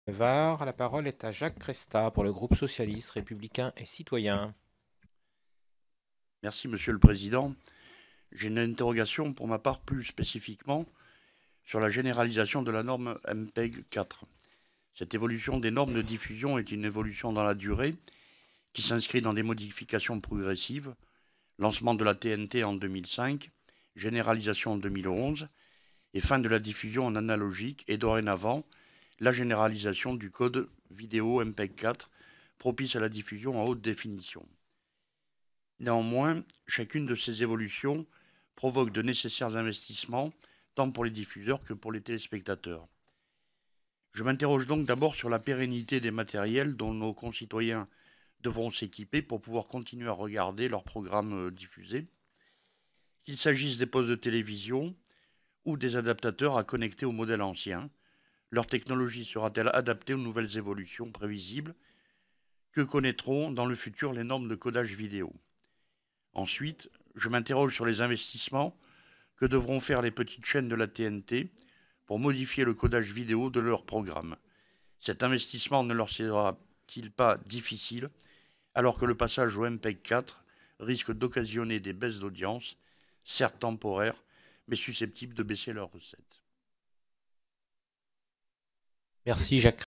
Modernisation de la télévision numérique terrestre
Le mercredi 17 juin 2015, en commission des Affaires culturelles et de l'éducation, Jacques CRESTA, député PS des Pyrénées Orientales, a interrogé Patrick BLOCHE, président de la commission, sur la proposition de loi relative au deuxième dividende numérique et à la poursuite de la modernisation de la télévision numérique terrestre, texte dont Patrick BLOCHE est le rapporteur.

"J’ai une interrogation plus spécifiquement sur la généralisation de la norme MPEG-4. Cette évolution des normes de diffusion est une évolution dans la durée, qui s’inscrit dans des modifications progressives : lancement de la TNT en 2005, généralisation en 2011 et fin de la diffusion en analogique, et dorénavant la généralisation du codage vidéo MPEG-4, propice à la diffusion en haute définition.

Néanmoins, chacune de ces évolutions provoquent de nécessaires investissements, tant pour les diffuseurs que pour les téléspectateurs.

Je m’interroge donc d’abord, sur la pérennité des matériels dont nos concitoyens devront s’équiper pour pouvoir continuer à regarder les programmes diffusés. Qu’il s’agisse des postes de télévisions ou des adaptateurs à connecter aux modèles anciens, leur technologie sera-t-elle adaptée aux nouvelles évolutions, prévisibles, que connaîtront les normes de codage vidéo ?

Ensuite, je m’interroge sur les investissements que devront faire les « petites » chaines de la TNT pour modifier le codage vidéo de leur programme. Cet investissement ne leur sera-t-il pas difficile alors que le passage au MPEG-4 risque d’occasionner des baisses d’audience, certes temporaires, mais susceptibles de baisser leurs recettes ?"